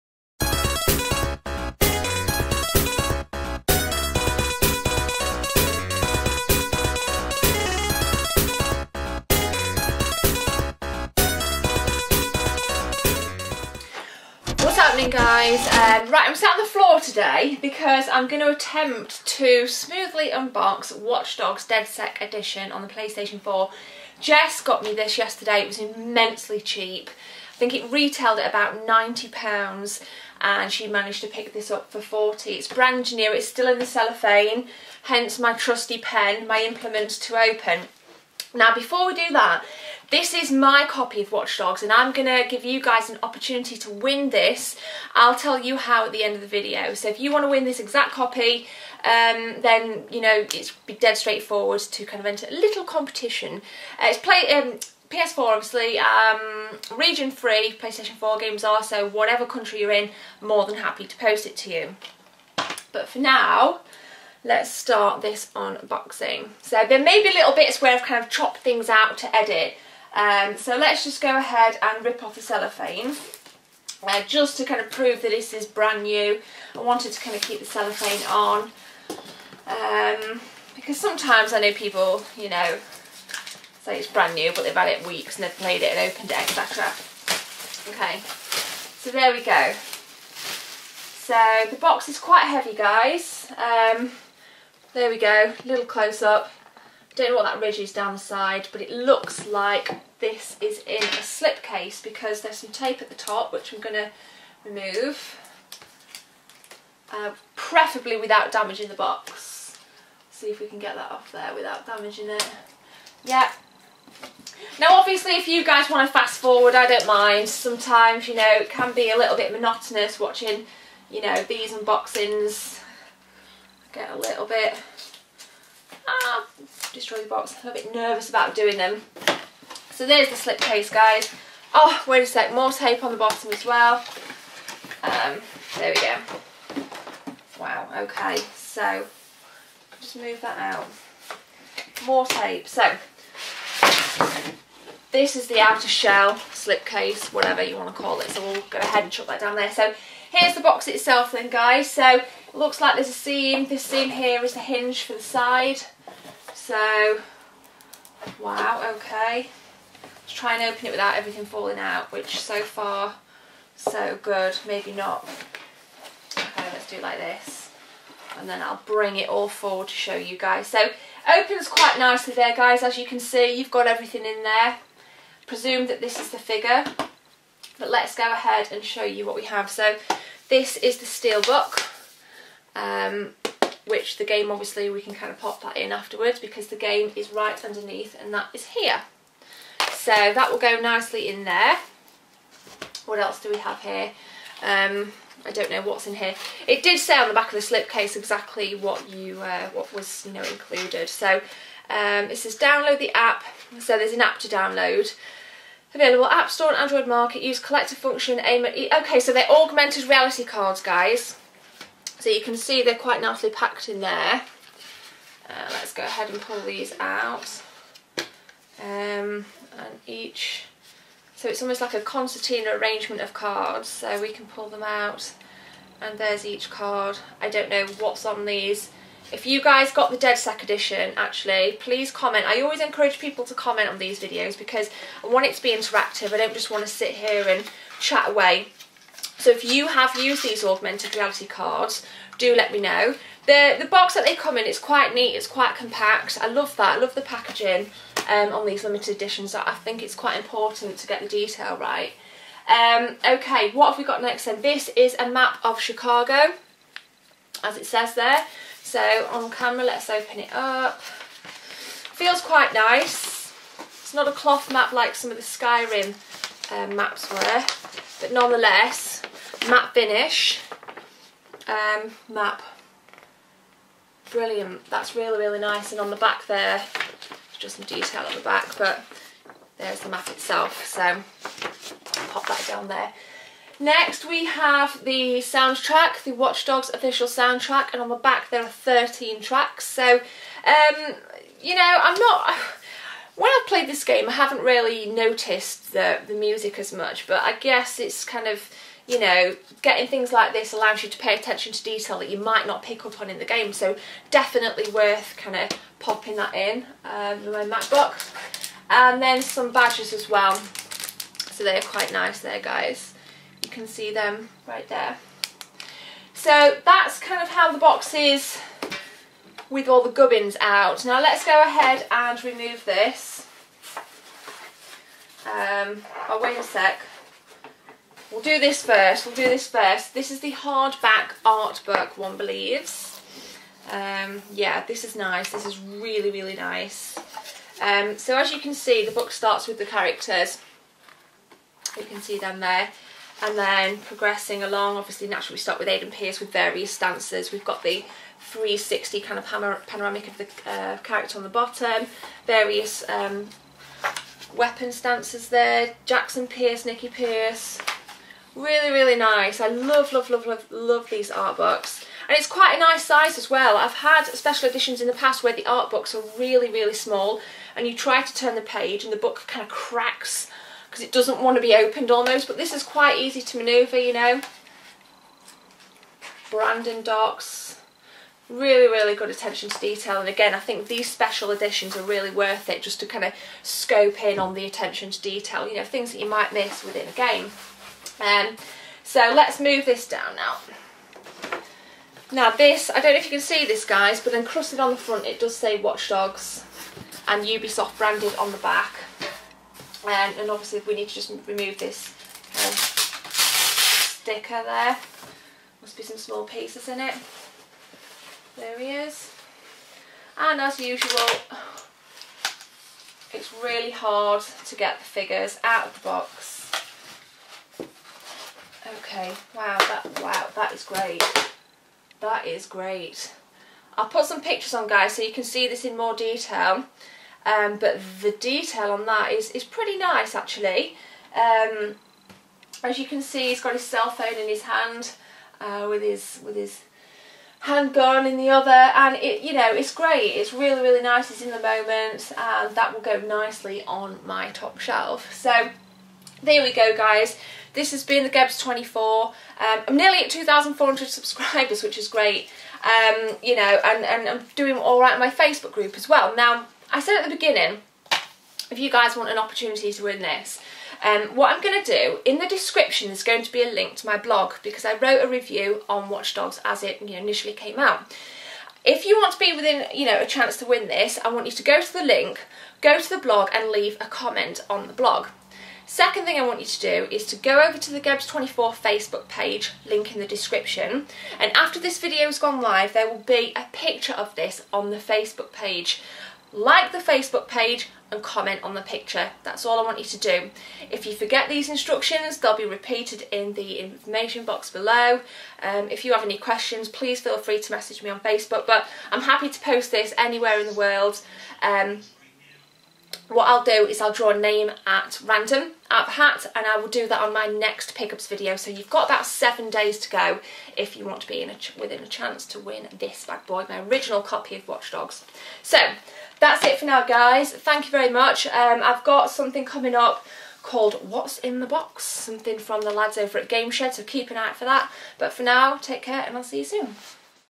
Girly Gamer: Watch Dogs Dedsec Unboxing PS4 and WIN a copy of Watchdogs
TO BE IN WITH A CHANCE TO WIN A COPY OF WATCHDOGS ON THE PS4 FOLLOW THESE INSTRUCTIONS:

3) COMMENT on the Watch Dogs competition picture on FB

A name will be drawn at random during my next pick up video.